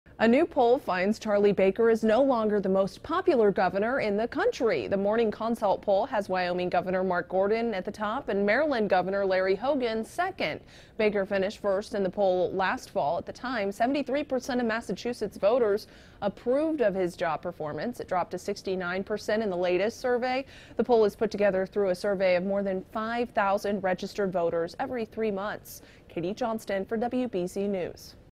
Charlie Baker Falls To Third In Most Popular Governor Poll, Wyoming's Mark Gordon First
Massachusetts Gov. Charlie Baker is no longer the most popular governor in the country, according to a new poll from Morning Consult. Katie Johnston reports.